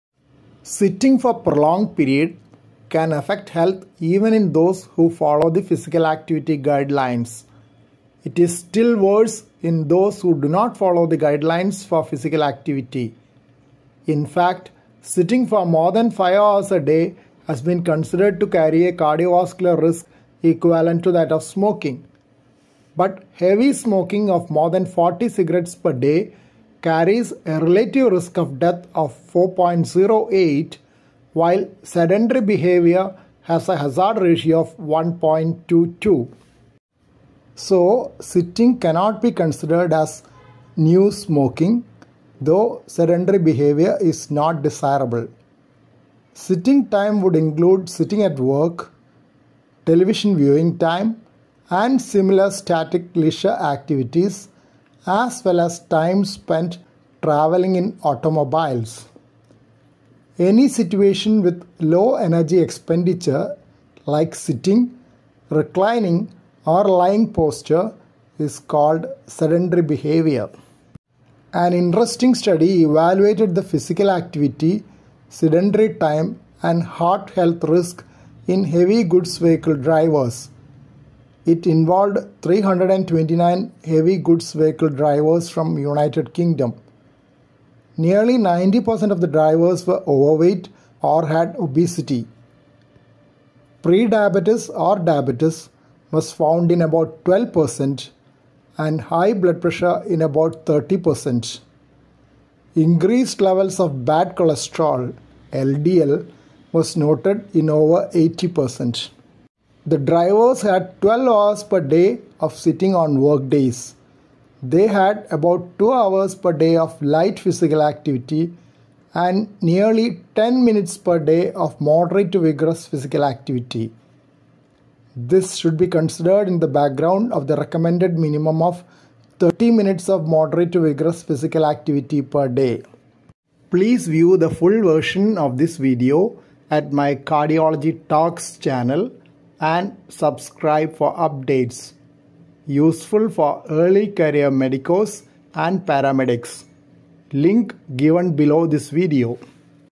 Is sitting for a long time so bad? Intro
Please view the full version of this video at my Cardiology Talks channel:
- Useful for early career medicos and paramedics
Sitting for prolonged period can affect health even in those who follow the physical activity guidelines. It is still worse in those who do not follow the guidelines for physical activity. In fact, sitting for more than five hours a day has been considered to carry a risk equivalent to that of smoking. But heavy smoking of more than 40 cigarettes per day carries a relative risk of death of 4.08, while sedentary behaviour has a hazard ratio of 1.22.